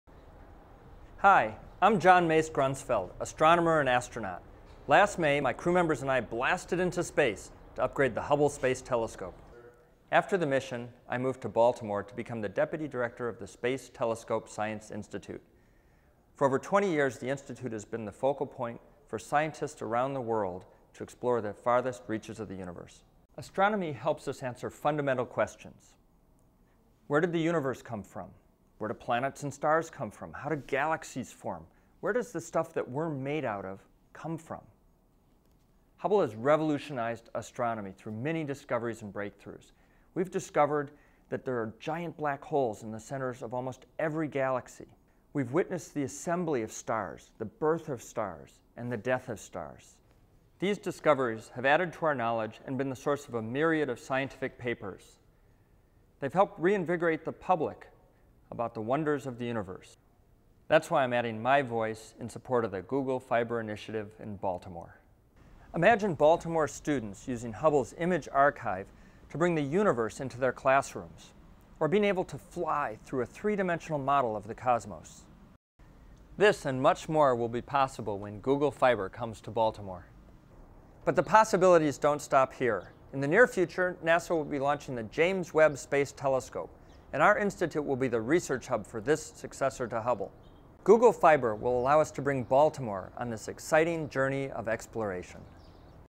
STScI supports Google Fiber
The Hubble Space Telescope Science Institute supports the Google Fiber initiative. John Mace Grunsfeld, astronaut, astronomer and deputy director of the Institute, showcases what the Google Fiber initiative could do for Baltimore.